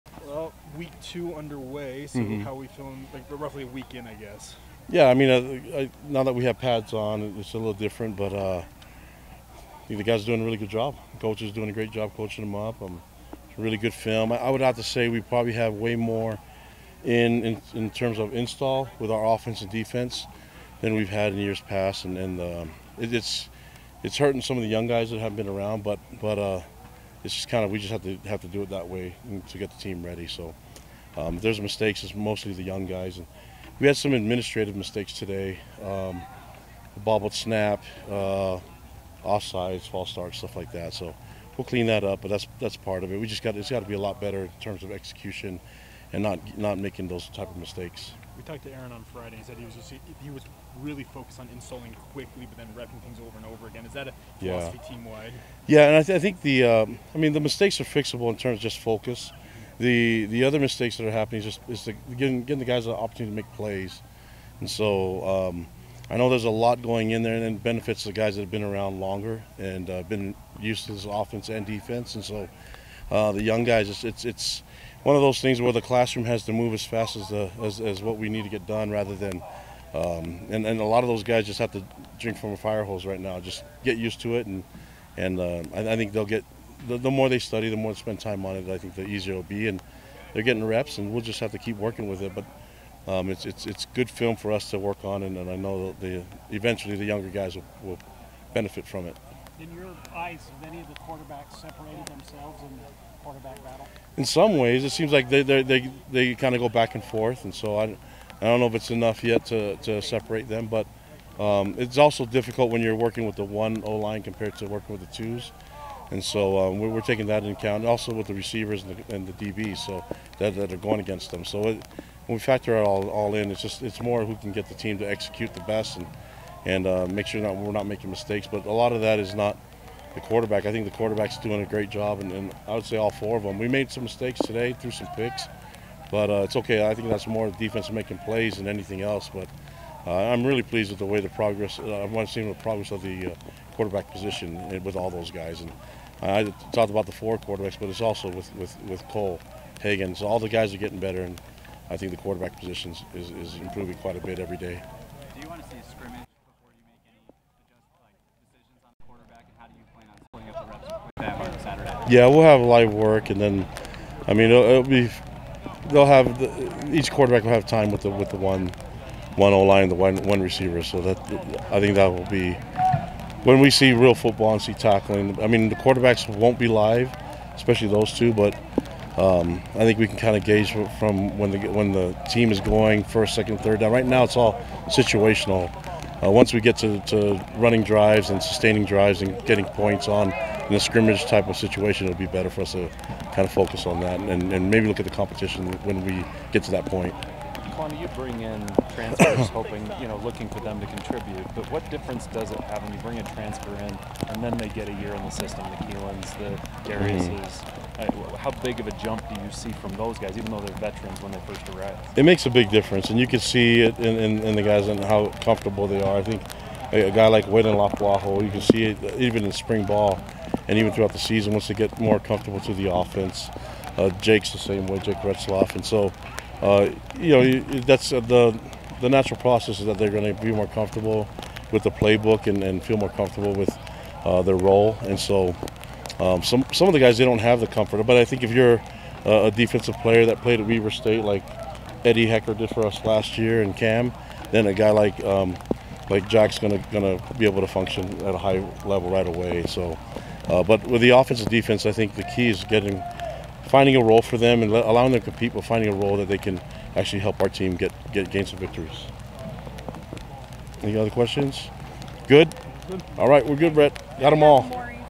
Kalani Sitake | Media Availability | Fall Camp | August 5, 2024
BYU Football Head Coach Kalani Sitake talks with the media during Fall Camp. August 5, 2024

Presented by Mountain America Credit Union